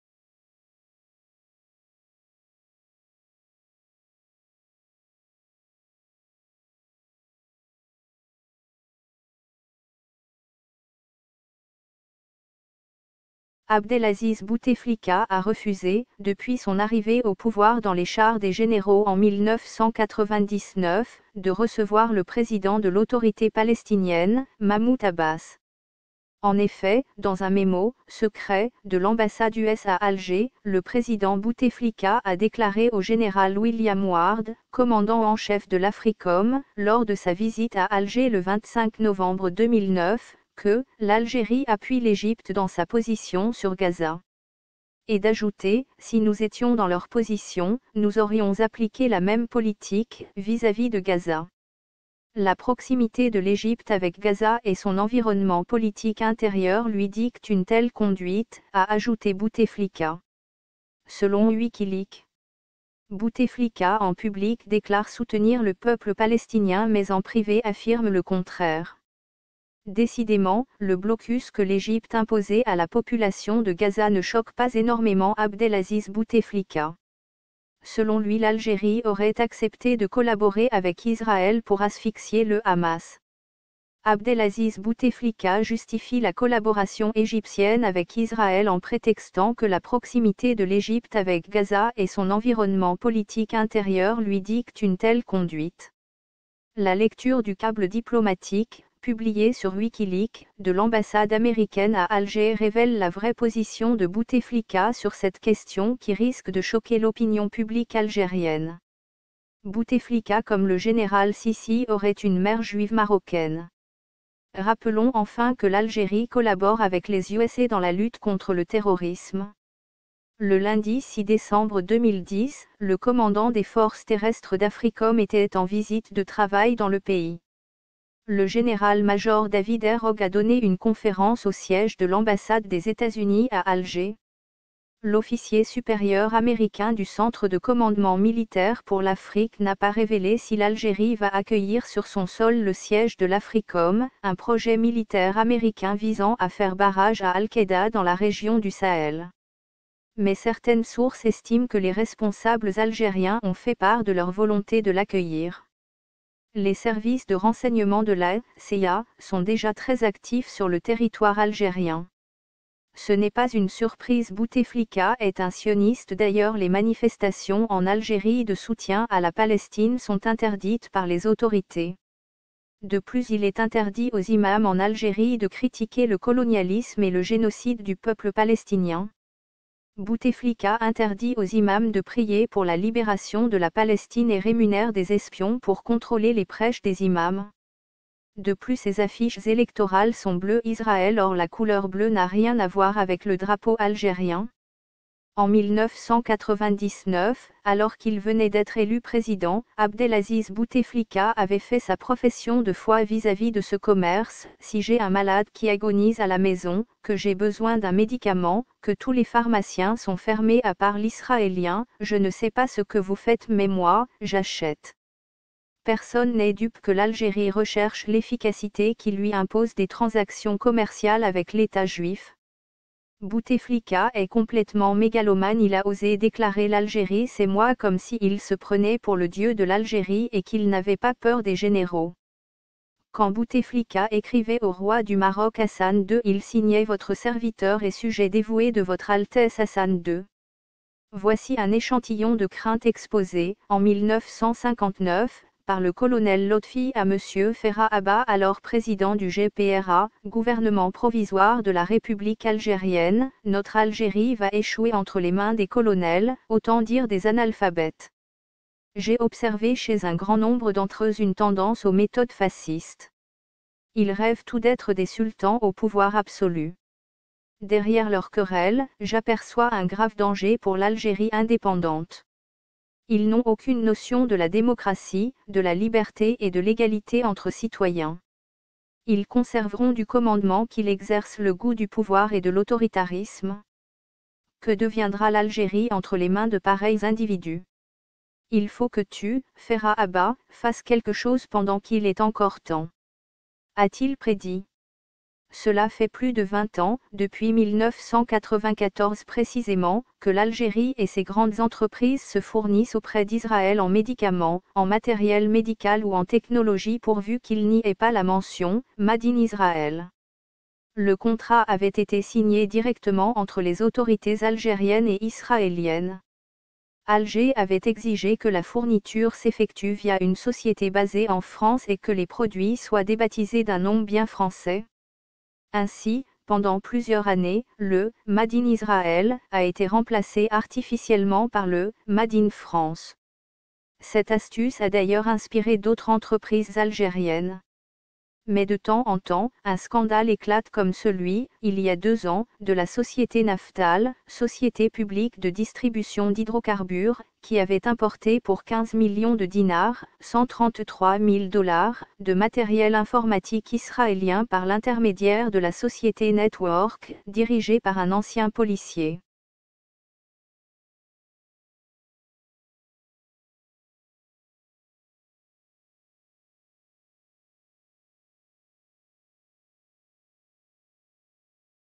Boutéflika soutient l'oppression du peuple palestinien par l'Egypte selon Wikileaks ?
Abdelaziz Bouteflika a refusé, depuis son arrivée au pouvoir dans les chars des généraux en 1999, de recevoir le président de l’autorité palestinienne, Mahmoud Abbas. En effet, dans un mémo « secret » de l’ambassade US à Alger, le président Bouteflika a déclaré au  général William Ward, commandant-en-chef de l’AFRICOM, lors de sa visite à Alger le 25 novembre 2009, que  »l’Algérie appuie l’Egypte dans sa position sur Ghaza ». Et d’ajouter: « Si nous étions dans leur position, nous aurions appliqué la même politique » vis-à-vis de Ghaza. « La proximité de l’Egypte avec Ghaza et son environnement politique intérieur lui dictent une telle conduite », a ajouté Bouteflika. Selon Wikileak.

Bouteflika en public déclare soutenir le peuple palestinien mais en privé affirme le contraire . Bouteflika est de mère juive marocaine comme Sissi.

Décidément, le blocus que l’Egypte imposé à la population de Gaza ne choque pas énormément Abdelaziz Bouteflika. Selon lui L’Algérie aurait accepté de collaborer avec Israël pour asphyxier le Hamas.

Abdelaziz Bouteflika justifie la collaboration égyptienne avec Israël en prétextant que “la proximité de l’Égypte avec Gaza et son environnement politique intérieur” lui dicte une telle conduite ! La lecture du câble diplomatique, publié sur Wikileaks, de l’ambassade américaine à Alger révèle la vraie position de Bouteflika sur cette question qui risque de choquer l’opinion publique Algérienne.

Rappelons enfin que l’Algérie collabore avec les USA dans la lutte contre le terrorisme.

L’officier supérieur américain du Centre de commandement militaire pour l’Afrique n’a pas révélé si l’Algérie va accueillir sur son sol le siège de l’Africom, un projet militaire américain visant à faire barrage à Al-Qaida dans la région du Sahel. Mais certaines sources estiment que les responsables algériens ont fait part de leur volonté de l’accueillir .

Ce n’est pas une surprise Boutéflika est un sioniste d’ailleurs les manifestations en Algérie de soutien à la Palestine sont interdites par les autorités. De plus il est interdit aux imams en Algérie de critiquer le colonialisme et le génocide du peuple palestinien. Boutéflika interdit aux imams de prier pour la libération de la Palestine et rémunère des espions pour contrôler les prêches des imams. De plus ses affiches électorales sont bleu Israël hors la couleur bleue n’as rien à voir avec le drapeau algérien.

En 1999, alors qu’il venait d’être élu président, Abdelaziz Bouteflika avait fait sa profession de foi vis-à-vis de ce commerce : «Si j’ai un malade qui agonise à la maison, que j’ai besoin d’un médicament, que tous les pharmaciens sont fermés à part l’israélien, je ne sais pas ce que vous faites mais moi, j’achète». Personne n’est dupe que l’Algérie recherche l’efficacité qui lui impose des transactions commerciales avec l’État juif.

Bouteflika est complétement mégalomane il a osé déclarer l’Algérie c’est moi comme si il se prenait pour le Dieu de l’Algérie et qu’il n’avait pas peur des généraux.

Quand Bouteflika écrivait au roi du Maroc Hassan 2 il signait  votre serviteur et sujet dévoué de votre altesse Hassan 2.

Voici un échantillon de craintes exposées, en 1959, par le colonel Lotfi à Monsieur Ferhat Abbas alors président du GPRA (gouvernement provisoire de la république algérienne) : « Notre Algérie va échouer entre les mains des colonels, autant dire des analphabètes. J’ai observé chez un grand nombre d’entre eux une tendance aux méthodes fascistes. Ils rêvent tous d’être des Sultans au pouvoir absolu. Derrière leurs querelles, j’aperçois un grave danger pour l’Algérie indépendante. Ils n’ont aucune notion de la démocratie, de la liberté et de l’égalité entre citoyens. Ils conserveront du commandement qu’ils exercent le goût du pouvoir et de l’autoritarisme. Que deviendra l’Algérie entre les mains de pareils individus ? Il faut que tu (Ferhat Abbas) fasses quelque chose pendant qu’il est encore temps… », a-t-il prédit.

Cela fait plus de vingt ans, depuis 1994 précisément, que l’Algérie et ses grandes entreprises se fournissent auprès d’Israël en médicaments, en matériel médical ou en technologie pourvu qu’il n’y ait pas la mention «made in Israël». Le contrat avait été signé directement entre les autorités algériennes et israéliennes.

Alger avait exigé que la fourniture s’effectue via une société basée en France et que les produits soient débaptisés d’un nom bien français. Ainsi, pendant plusieurs années, le «made in Israël» a été remplacé artificiellement par le «made in France». Cette astuce a d’ailleurs inspiré d’autres entreprises algériennes.

Mais bien sûr les échanges commerciaux ont été à double sens. La Sonatrach livrait du gaz algérien à Israël via le gazoduc d’El-Arish depuis l’Égypte. Le contrat liait les deux parties depuis le 1er août 2014. L’Égypte importait 500 millions de m3 de gaz par jour, en gardait 400 pour ses besoins et en revendait 100 à Israël.